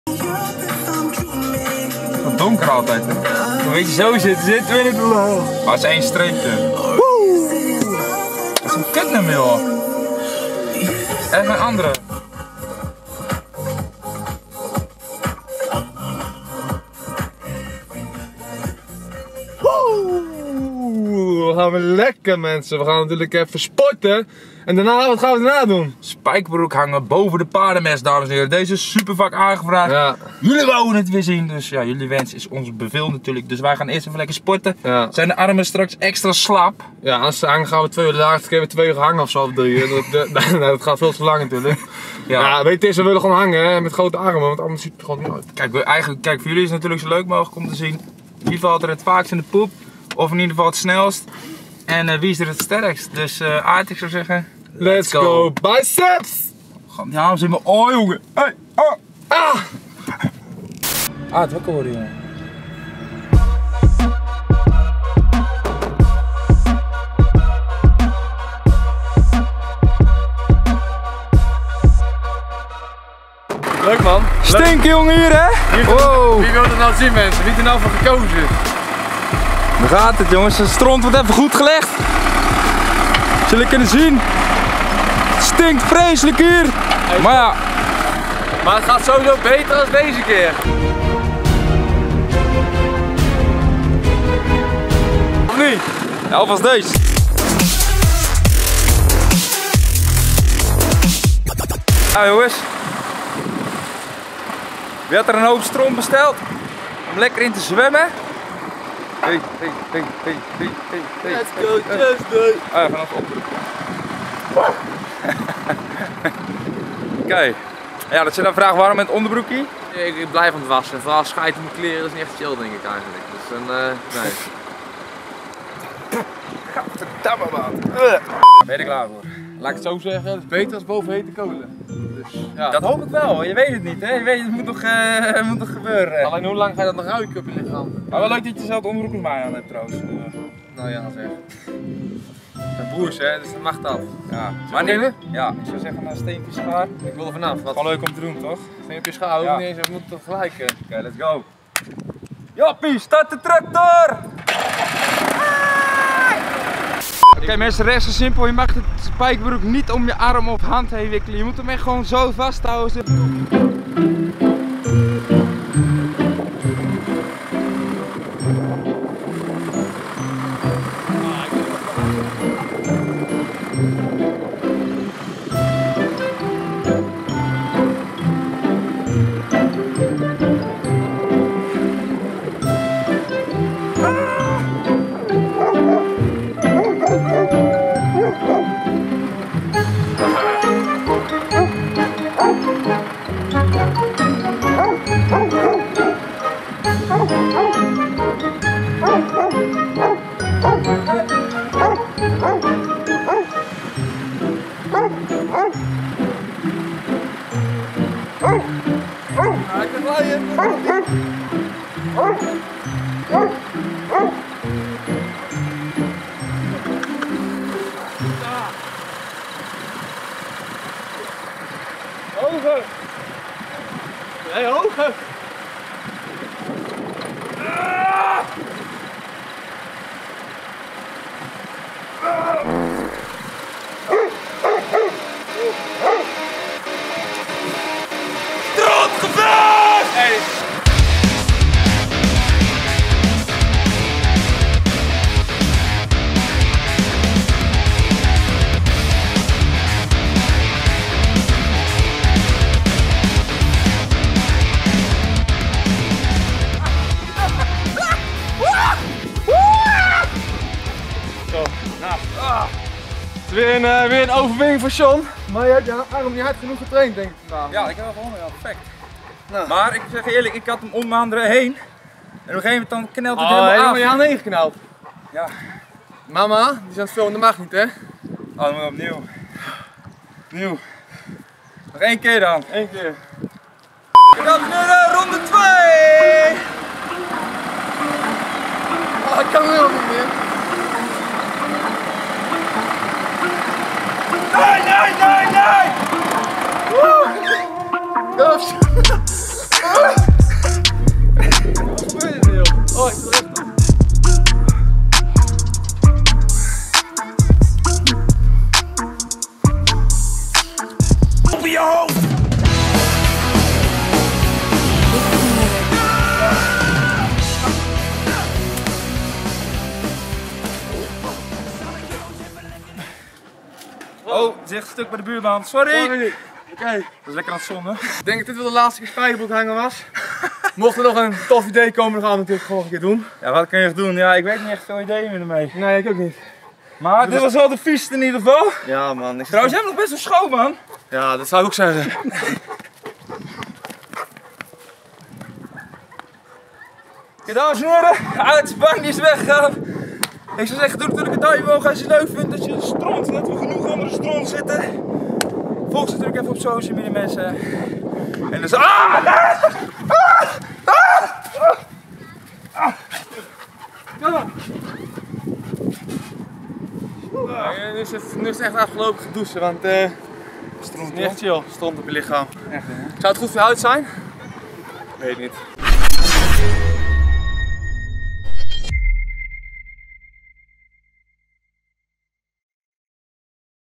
SPIJKERBROEK HANGEN BOVEN POEP BERG!
In de video van vandaag gaan Johnquote & Aarthos een wedstrijdje doen wie het sterkst is. De regels zijn simpel wie het langst blijft hangen aan de spijkerbroek wint! in het verleden hebben we al verschillende type spijkerbroekhangen gedaan zoals: Spijkerbroek hangen boven hete kolen & boven IJS water.